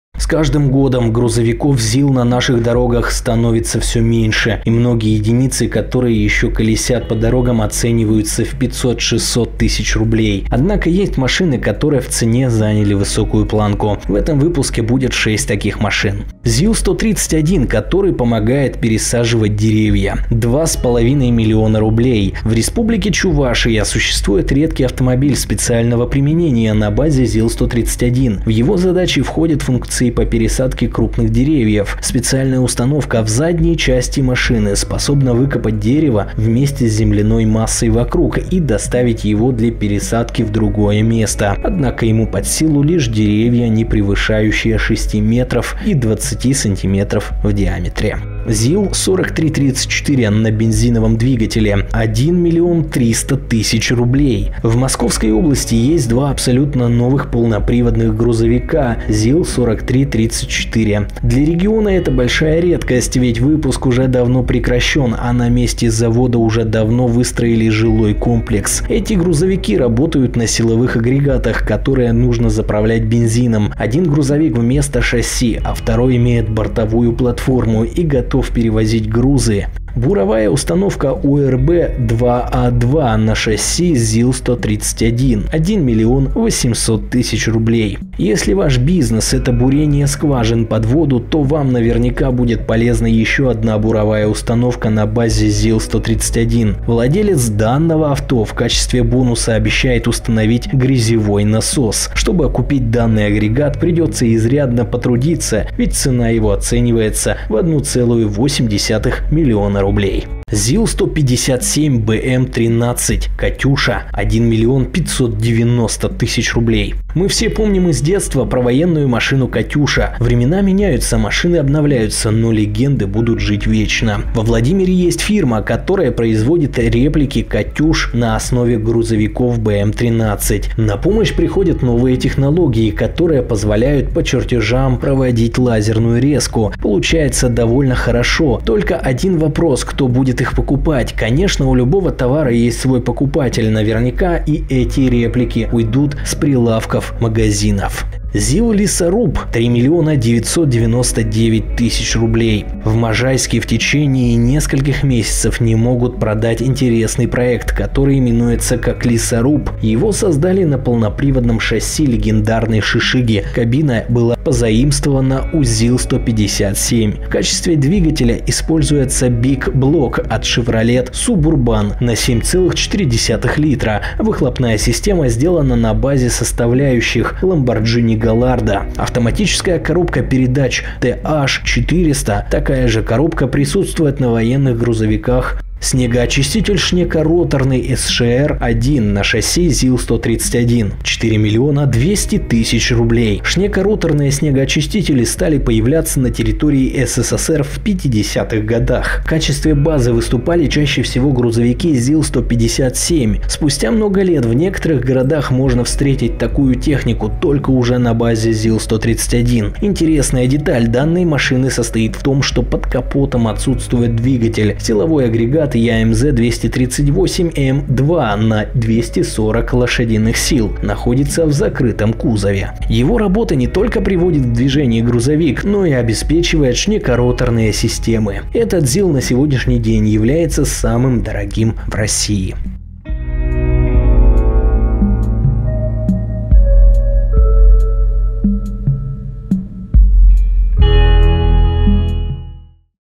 5 самых дорогих ЗИЛ из СССР, которые сейчас продаются в России!
5 самых дорогих ЗИЛ из СССР, которые сейчас продаются в России!